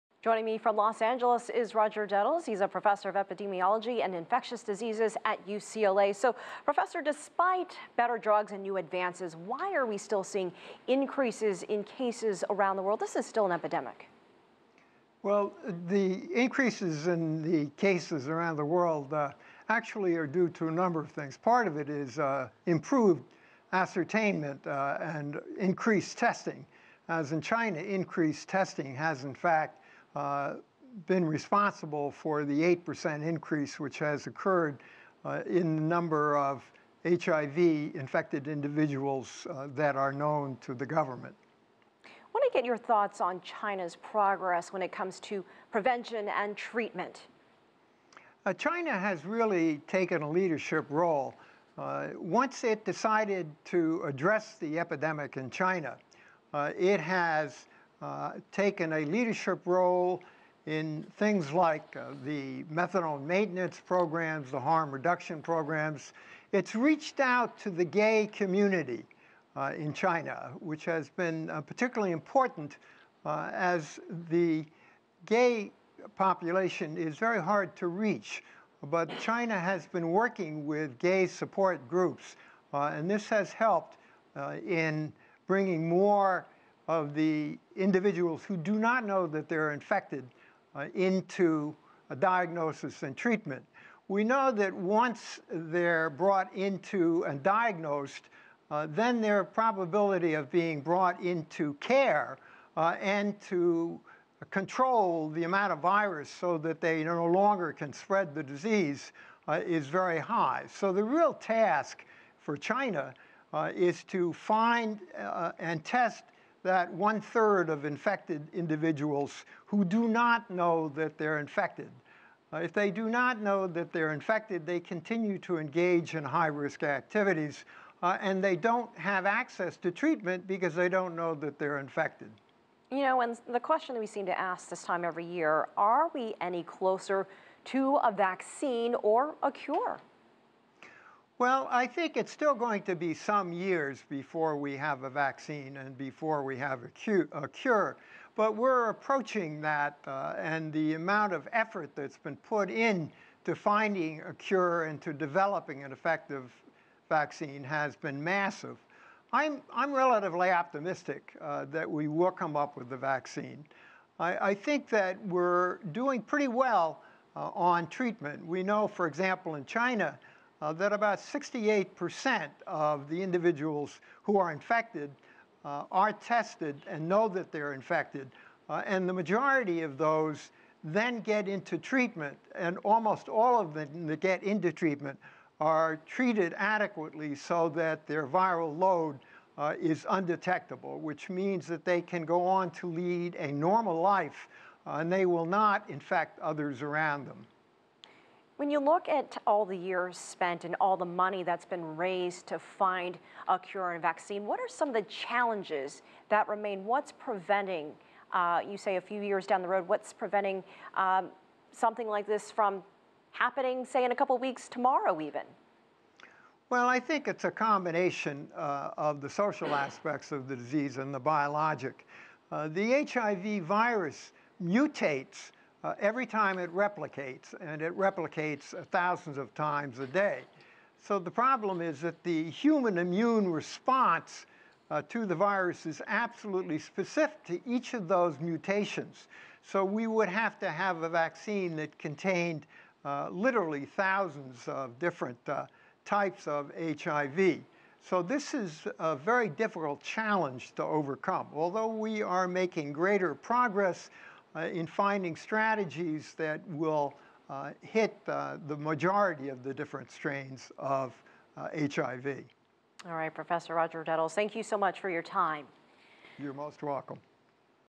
Roger Detels discusses HIV in China and around the world
HIV remains a stigma in China, but some groups are taking steps to change this. A popular dating app and Chinese health officials recently teamed up to increase awareness and promote HIV testing. Roger Detels, a professor of epidemiology and infectious diseases at UCLA, discusses these efforts and the worldwide fight with CGTN’s Elaine Reyes.